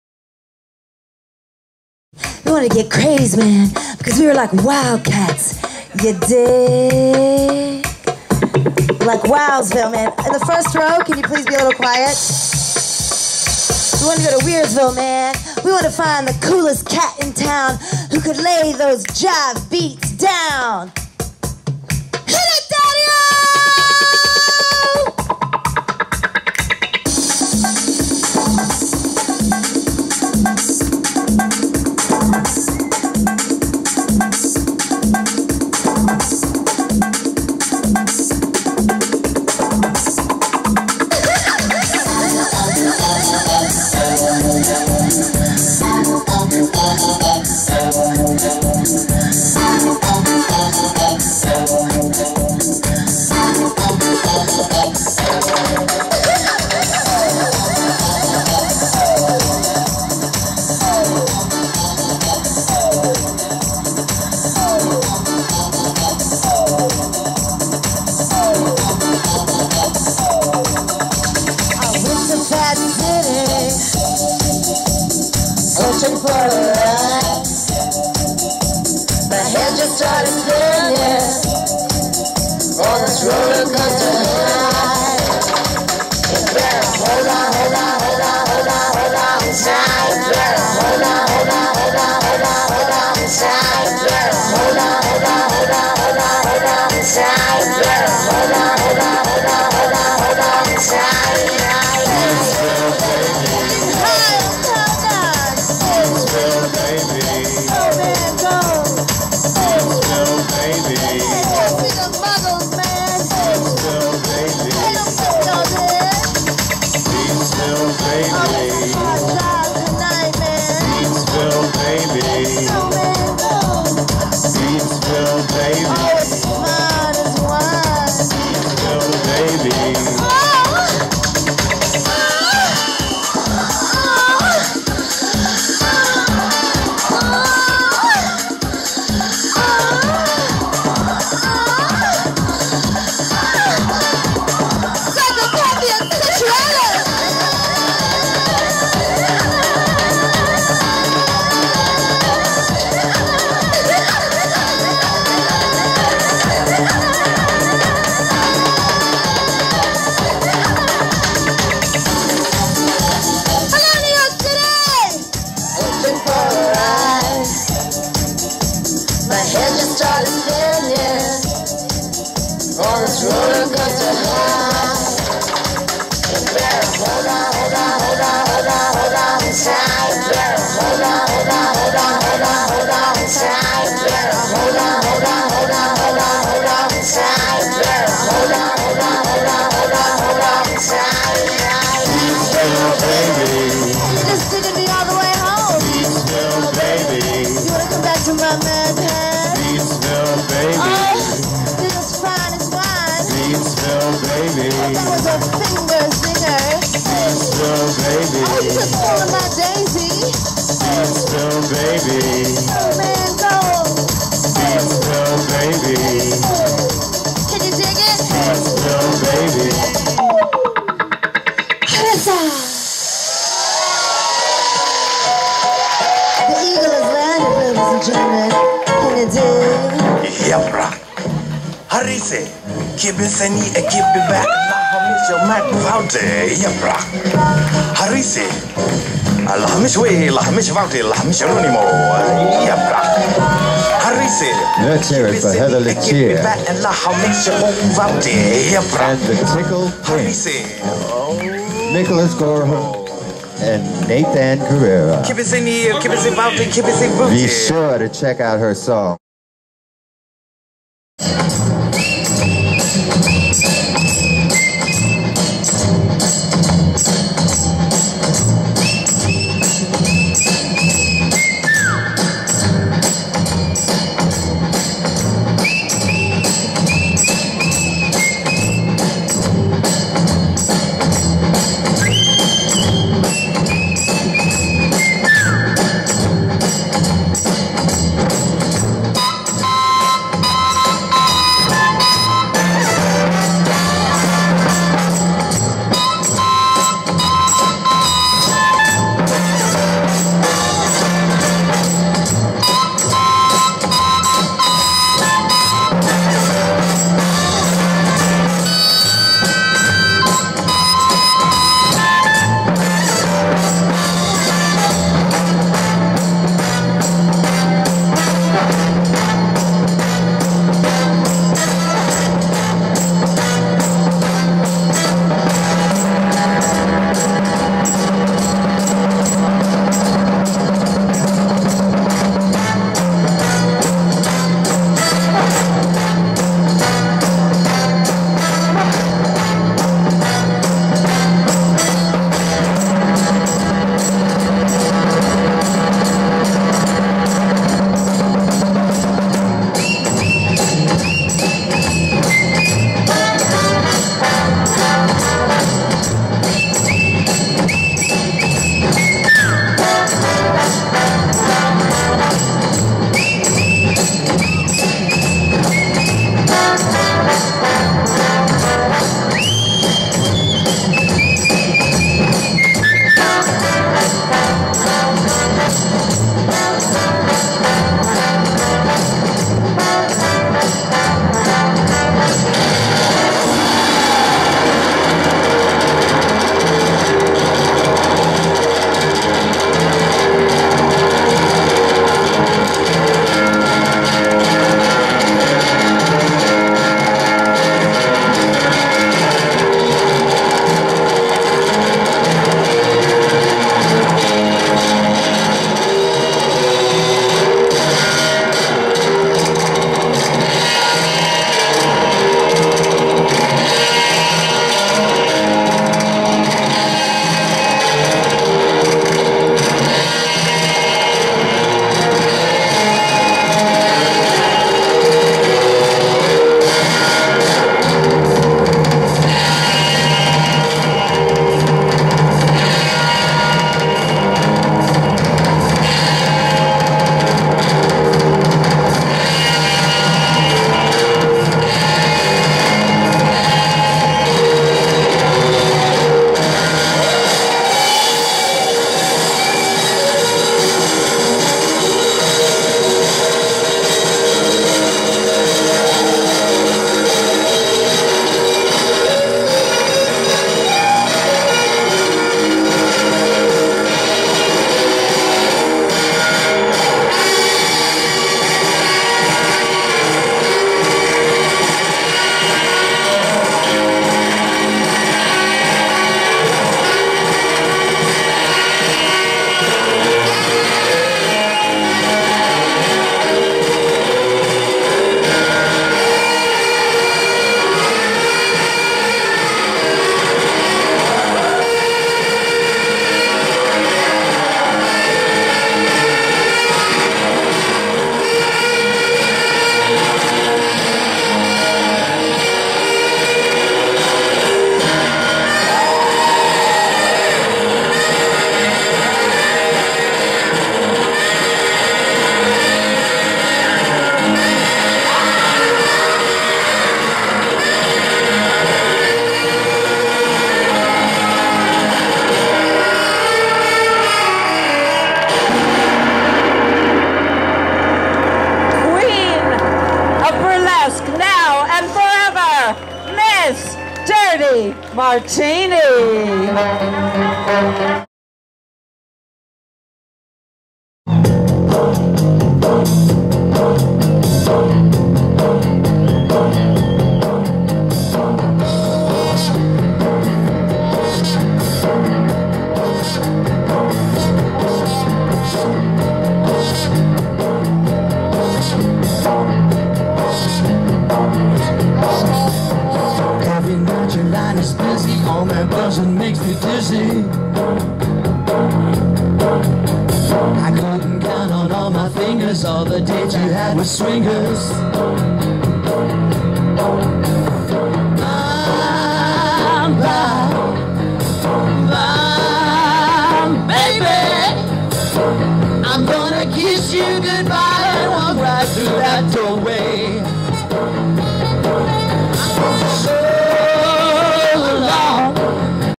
Best video of 'Low Life 4:Beat girl' of HOWL festival 2010(2/3)-----new york videodyssey(146)
'Ms. orgasm' Heather Litteer was doing her best performance,she is sexy as hell! Dirty Martini is as good as always and sherry vine & joey Arias was smart to bring a cute boy on stage with them.——纽约启视录2010